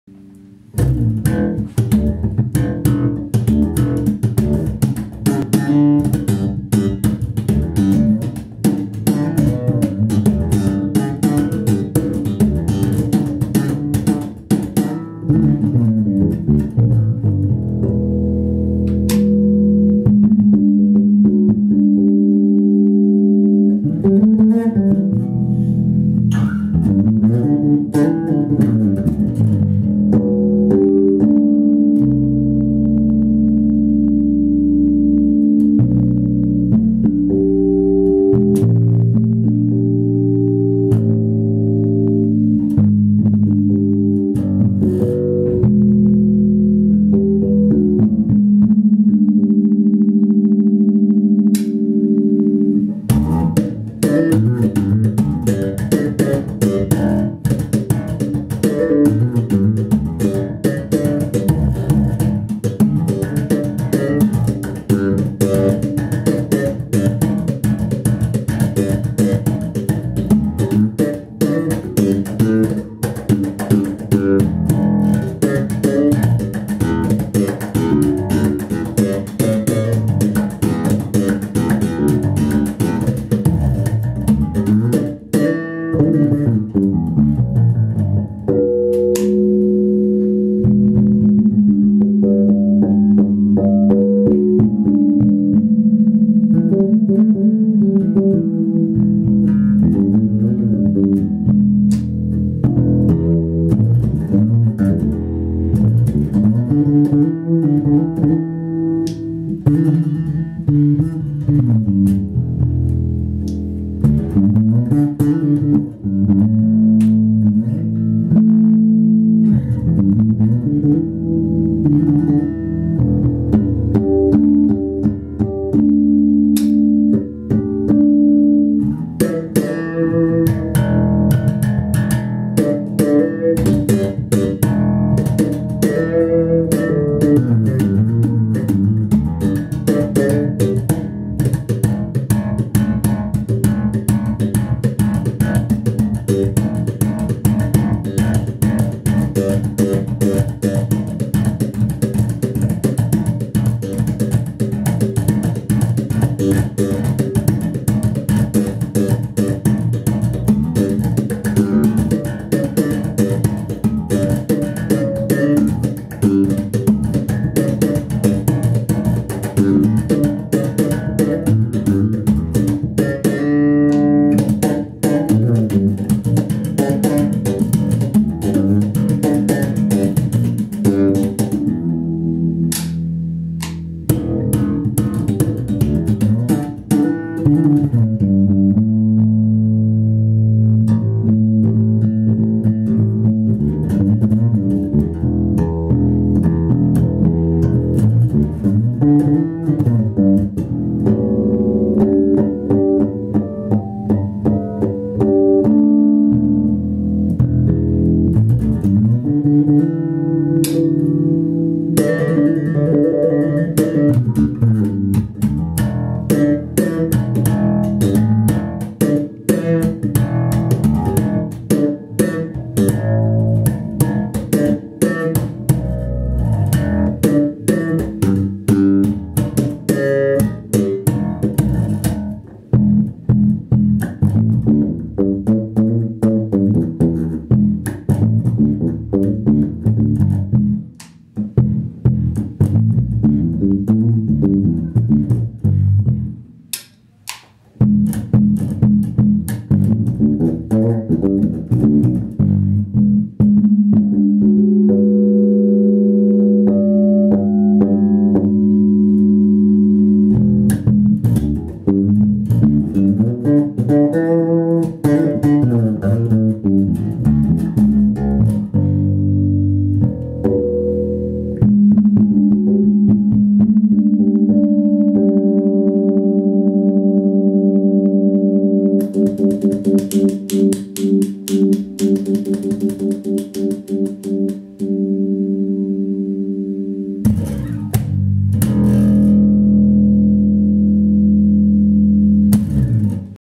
BC Rich Mocking Bird Bass repaired by Ryuo Motoyama
neck,bridge,octave,pu balance,string tension control,nut,adj
passive mod,noise shield
EXL-165